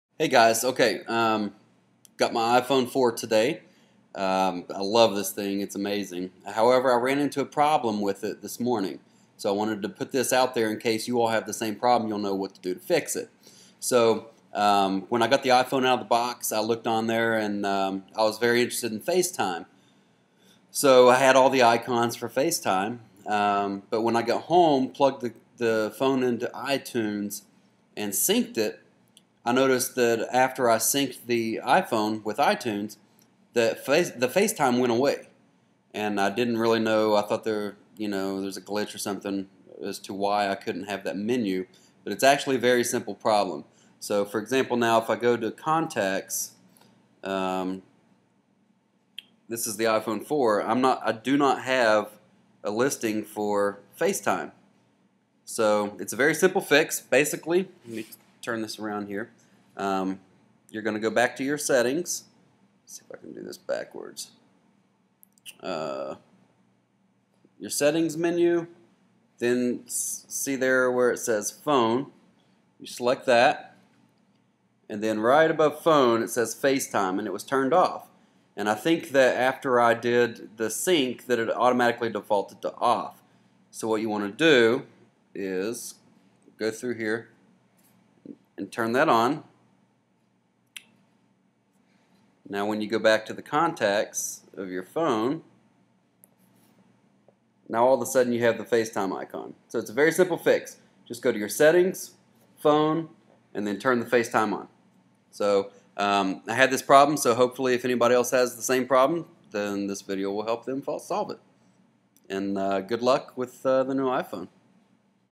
iPhone4 FaceTime not working after sync?
My FaceTime worked fine until I synced, then it stopped working. Easy fix in the phone settings.

Settings - Phone - Facetime - On

For some reason, syncing set the default to "off".